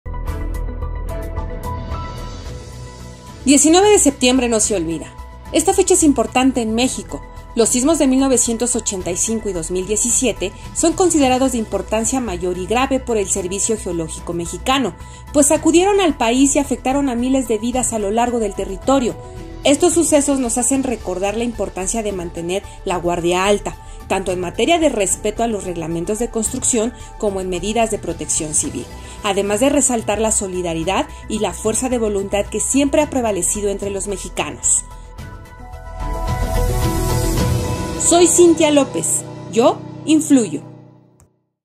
19 de septiembre no se olvida
Los terremotos de 1985 y 2017 que azotaron a la República Mexicana marcaron la vida de las personas que vivieron estas tragedias.